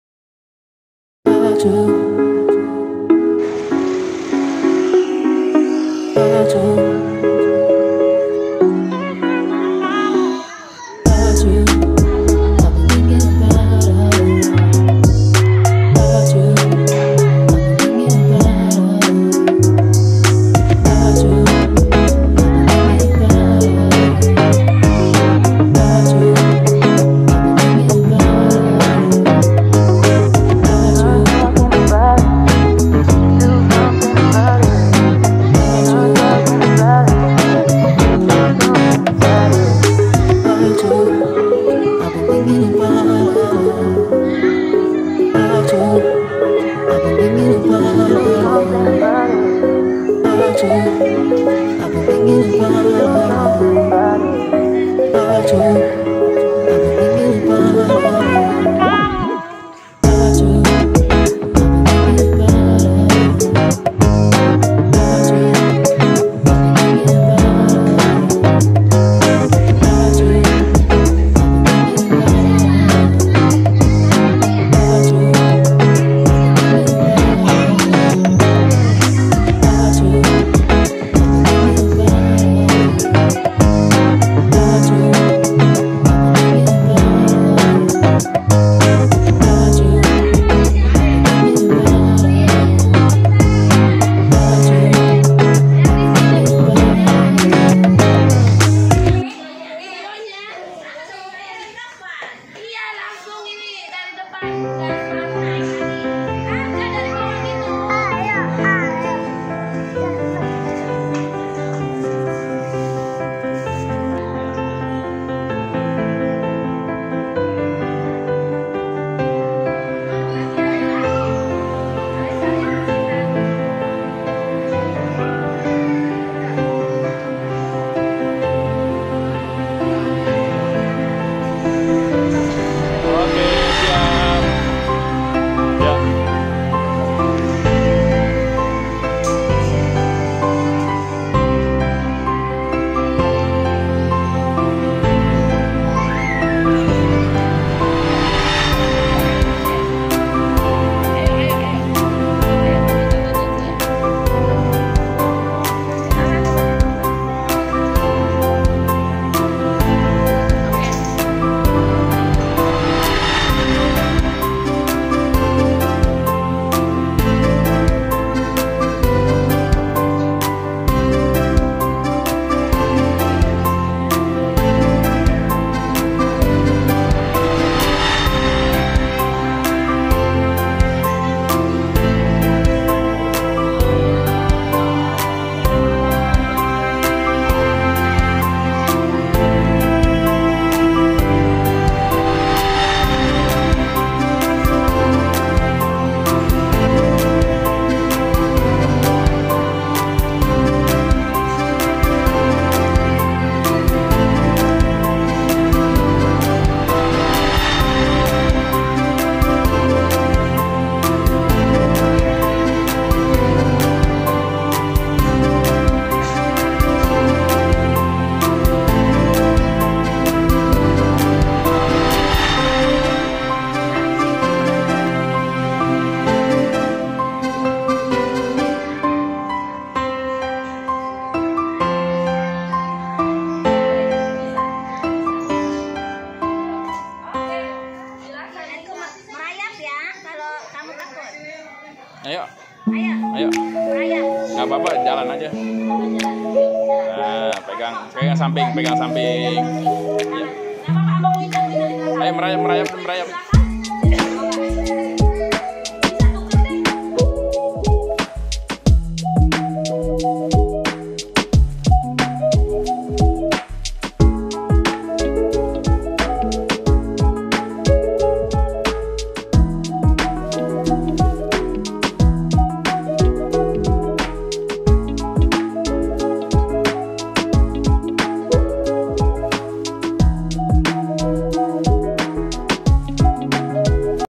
Outbound Shidqia islamic preschool Jati asih - kota Bekasi
kegiatan outbound sangatlah penting untuk menunjang proses belajar . dengan membantu motorik kasar ( merayap, melompat , menegang) melatih keberanian dan jiwa pemimpin

Shidqia islamic preschool punya wahana outbound  sendiri di lingkungan sekolah jadi kapanpun anak didik bisa bermain jarang jarang kan sekolah punya wahana outbound

teruntuk kawan kawan ayah bunda yg tinggal di wilayah jati asih untamanya wilayah AURI dan sekitarnya  yg mau cari sekolah untuk buah hatinya saya rekomendasikan di sini saja
alamatnya

Jl. Surya Dharma, AV.8, perum Bumi dirgantara, RT.001/RW.003, Jatisari, Kec. Jatiasih, Kota Bks, Jawa Barat 17415